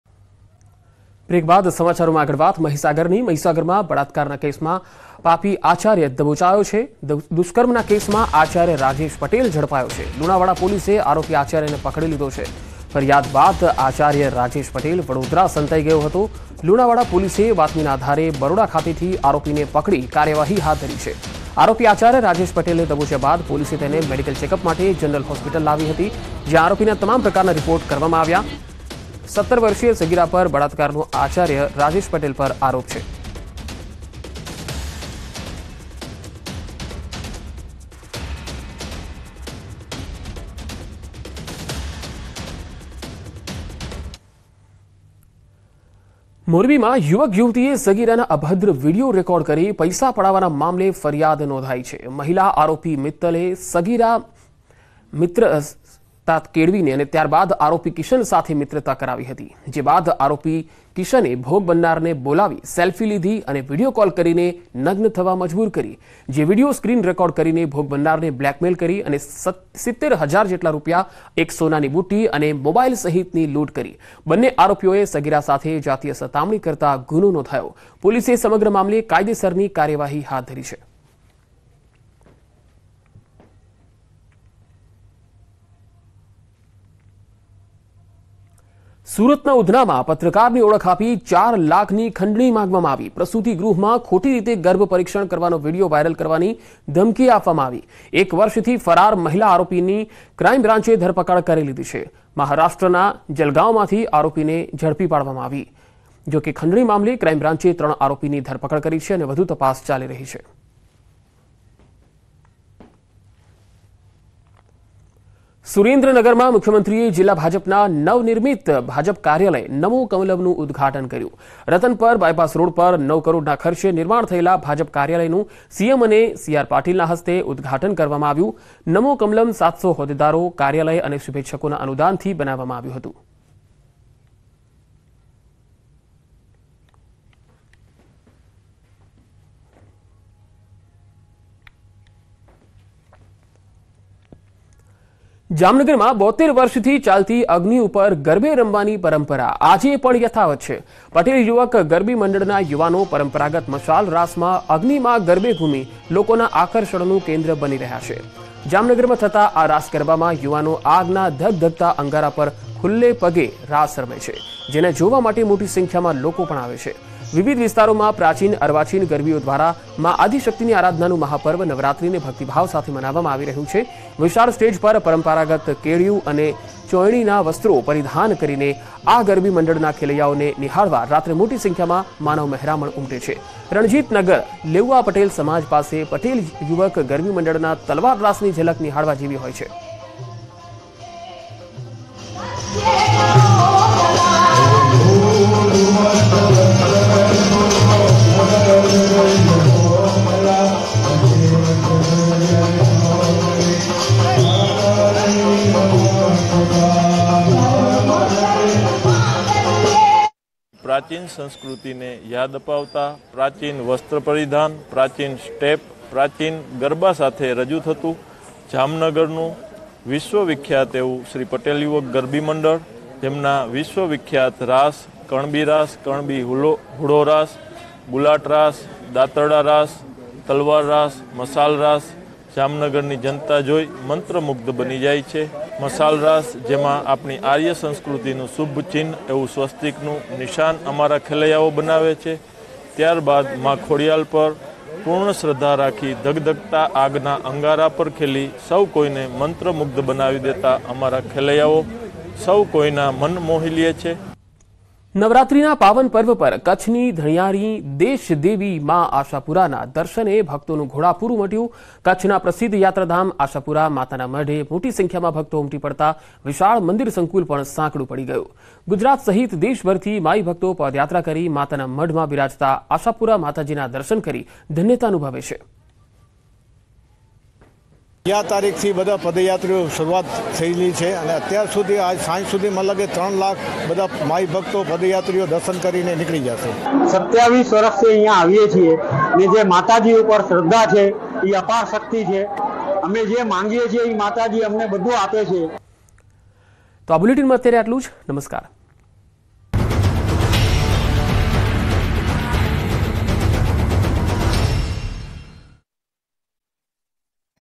Breaking News | મહીસાગર દૂષ્કર્મ કેસમાં પાપી આચાર્ય દબોચાયો | Crime News |Gujarat Police |News18
Papi Acharya arrested in rape case in Mahisagar. Acharya Rajesh Patel arrested in rape case. Lunawada Police arrested the accused Acharya. After the complaint, Acharya Rajesh Patel was hiding in Vadodara.

મહીસાગરમાં બળાત્કાર કેસમાં પાપી આચાર્ય દબોચાયો. દુષ્કર્મ કેસમાં આચાર્ય રાજેશ પટેલ ઝડપાયો. લુણાવાડા પોલીસે આરોપી આચાર્યને પકડ્યો. ફરિયાદ બાદ આચાર્ય રાજેશ પટેલ વડોદરા સંતાયો હતો.n18oc_mahisagar